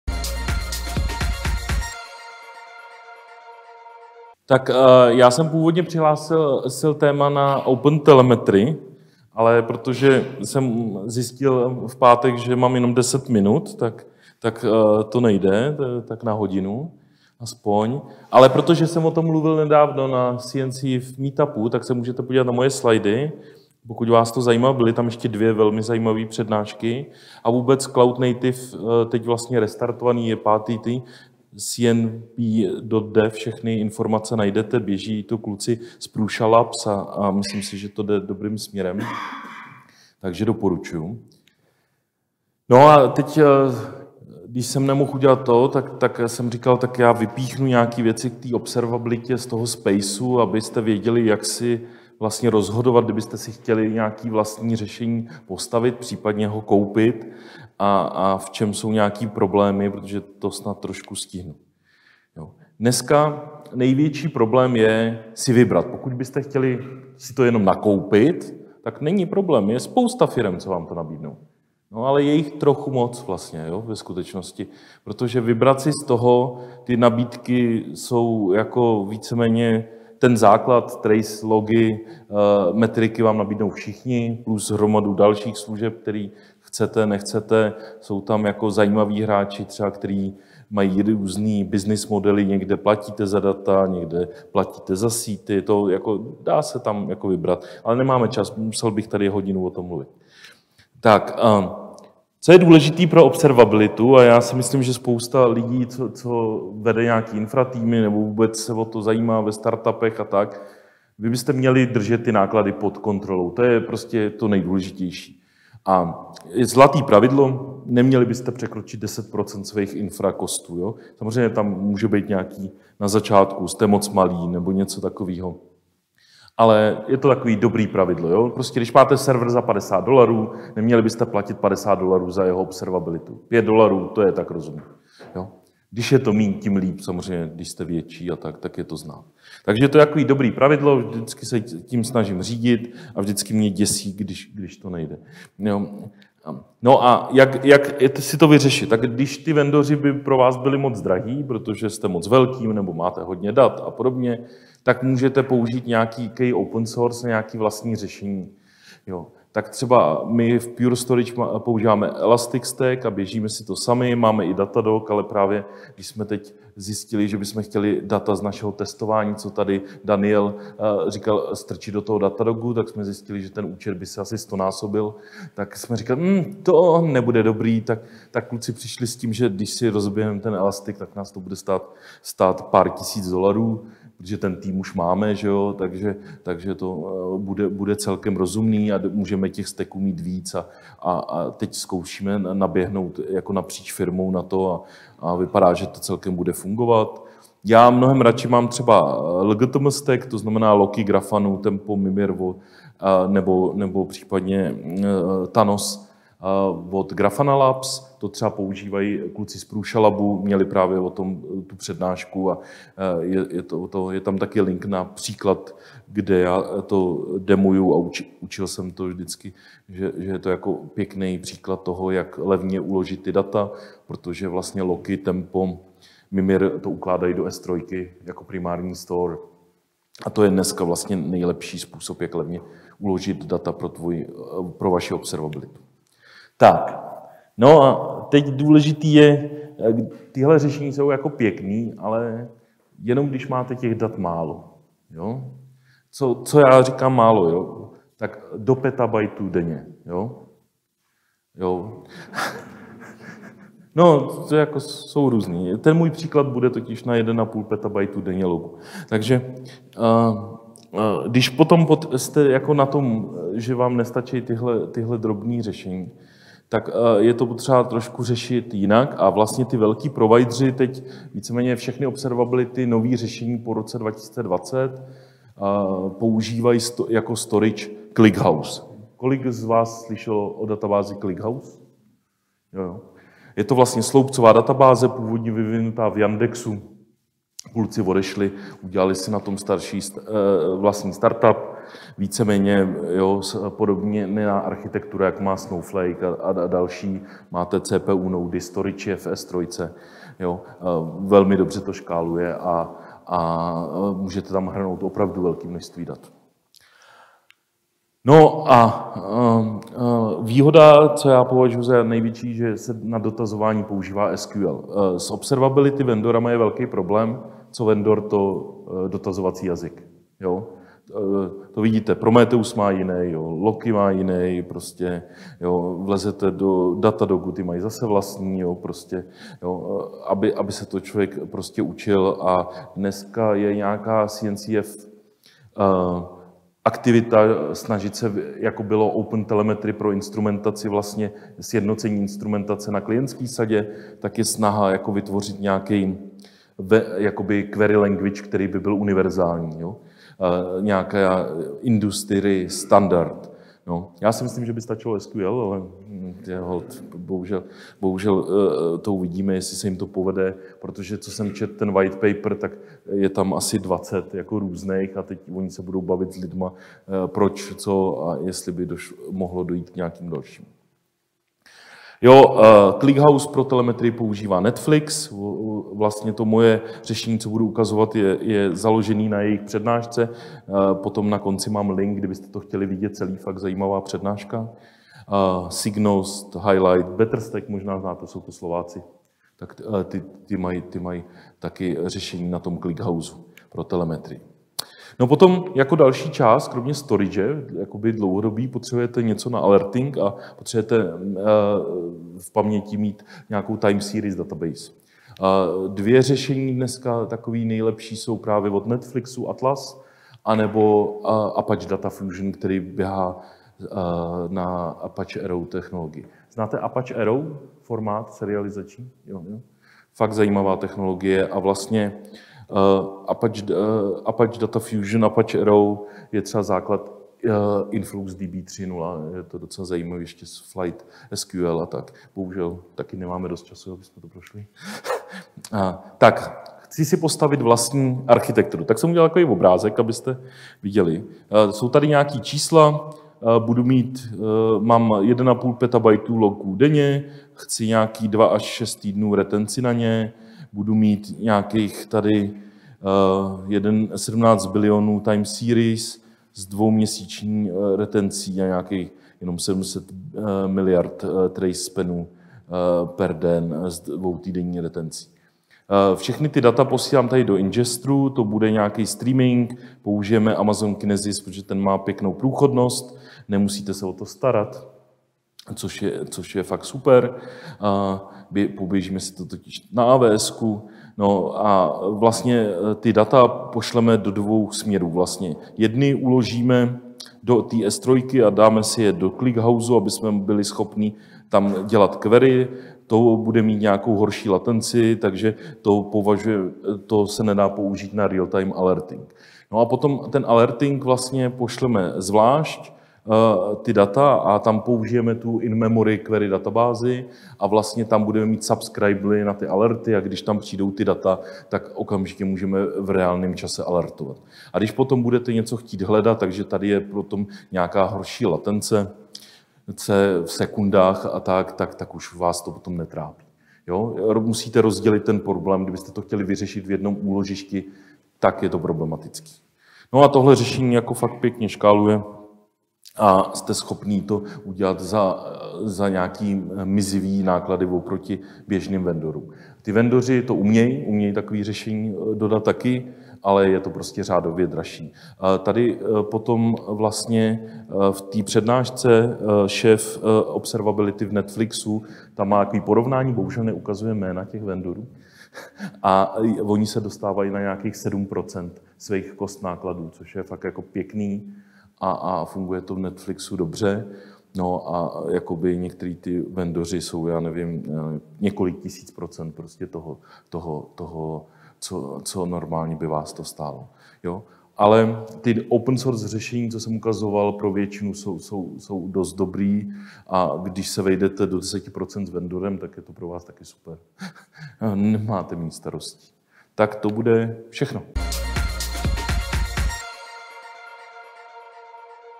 jOpenSpace 2023 - Ladislav Prskavec - Jak si navrhnout vlastní observability backend
- observability řeší příliš mnoho firem
- náklady na observabilitu by obvykle neměli přesáhnout 10% nákladů na infrastrukturu
- doporučuje LGTM stack  - Loki, Grafana, Tempo a Mimir
- Clickhouse - sloupcová databáze, používá SQL pro dotazování
time-series databáze - doporučuje: Netflix Attlas, Apache DataFusion
- Pipeline:
   - Amazon Kinesis Ingestor
   - uložení do S3
   - transformační logika
   - ClickHouse
- vedle toho provozovat TS databázi, která pomůže řešit alerting díky In Memory TSDB